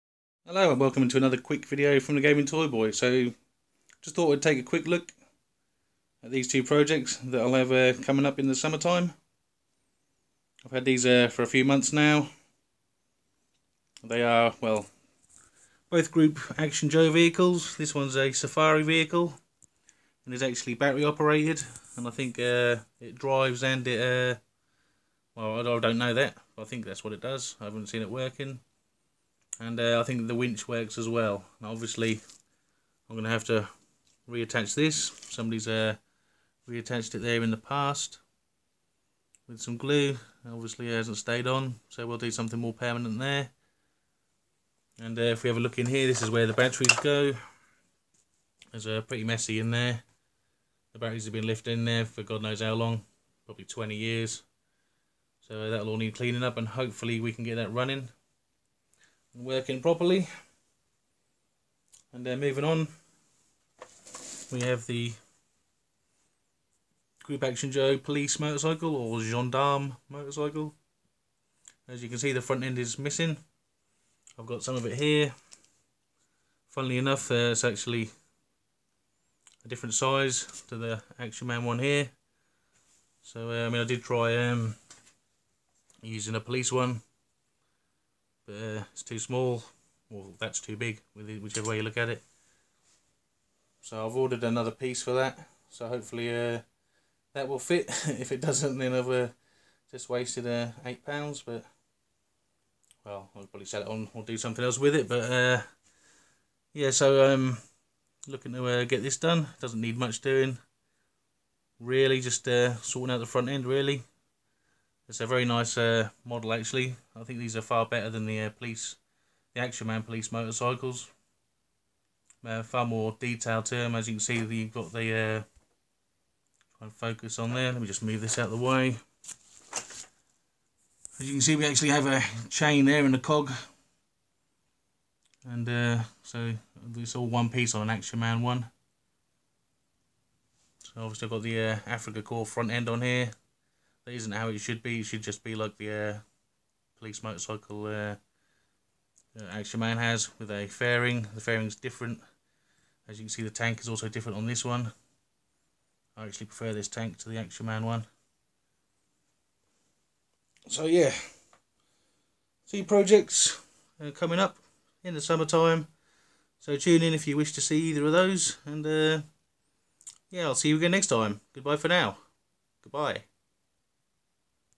Group Action Joe. projects for the summer.
Picked these up from ebay france, i just had to have that motorcycle, even though I had to pay more than I wanted to.